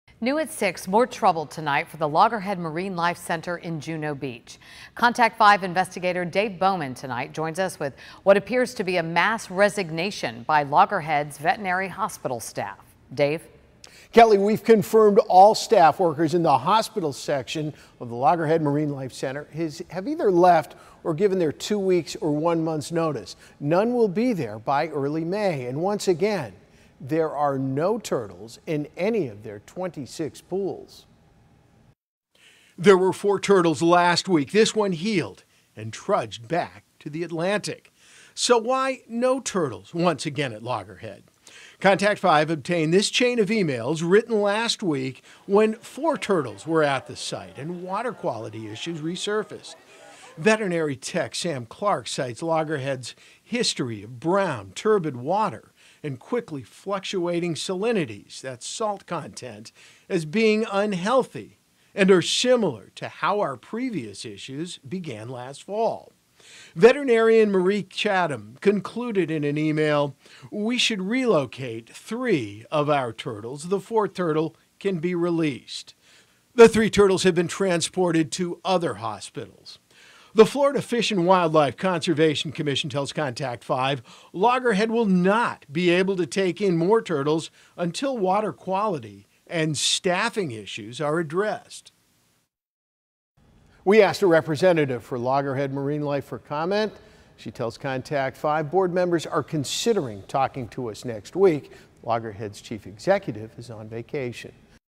Mass resignation of hospital staff at Loggerhead Marinelife Center
Ongoing water quality issues and now staffing issues are impacting Loggerhead Marinelife Center in Juno Beach.